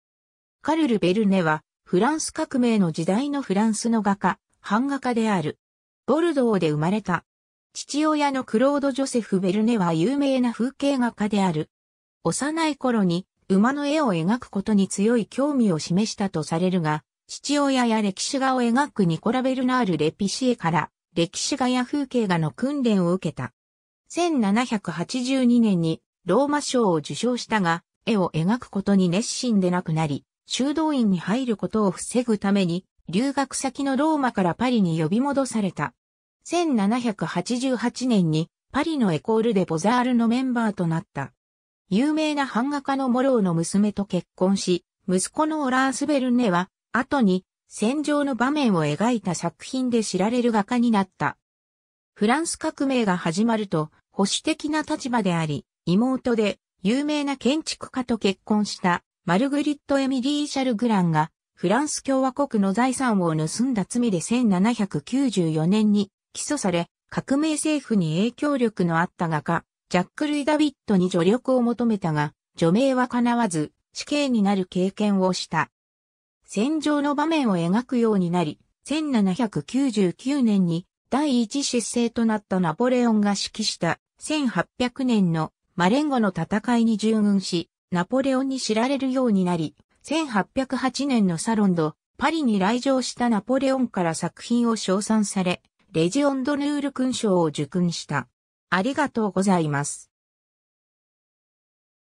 カルル・ヴェルネ
カルル・ヴェルネ（Carle Vernet、本名 Antoine Charles Horace Vernet、1758年8月14日 - 1836年11月17日）はフランス革命の時代のフランスの画家、版画家である。
 ボルドーで生まれた。
父親のクロード・ジョセフ・ヴェルネは有名な風景画家である。
幼い頃に馬の絵を描くことに強い興味を示したとされるが、父親や歴史画を描くニコラ・ベルナール・レピシエから歴史画や風景画の訓練を受けた。
1782年にローマ賞を受賞したが、絵を描くことに熱心でなくなり、修道院に入ることを防ぐために、留学先のローマからパリに呼び戻された。
1788年にパリのエコール・デ・ボザールのメンバーとなった。
有名な版画家のモロー(Jean-Michel Moreau)の娘と結婚し、息子のオラース・ヴェルネは後に戦場の場面を描いた作品で知られる画家になった。
 フランス革命が始まると、保守的な立場であり、妹で、有名な建築家と結婚したマルグリット・エミリー・シャルグラン(Marguerite Émilie Chalgrin)がフランス共和国の財産を盗んだ罪で1794年に起訴され、革命政府に影響力のあった画家、ジャック＝ルイ・ダヴィッドに助力を求めたが、助命はかなわず、死刑になる経験をした。
 戦場の場面を描くようになり、1799年に第一執政となったナポレオンが指揮した、1800年のマレンゴの戦いに従軍し、ナポレオンに知られるようになり、1808年のサロン・ド・パリに来場したナポレオンから作品を称賛され、レジオンドヌール勲章（シュバリエ）を受勲した 。